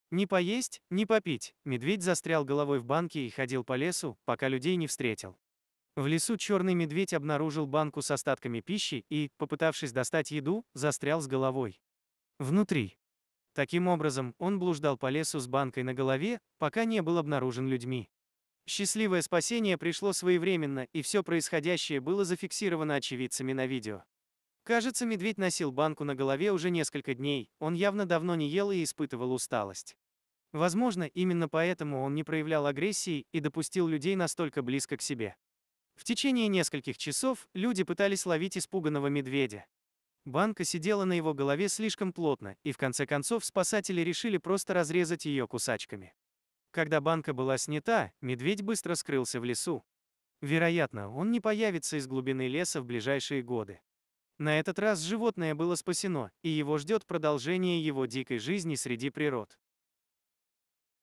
У Медведя голова застряла в Бочке, ни поесть ни попить
У Медведья голова застряла в Бочке, не поесть не попить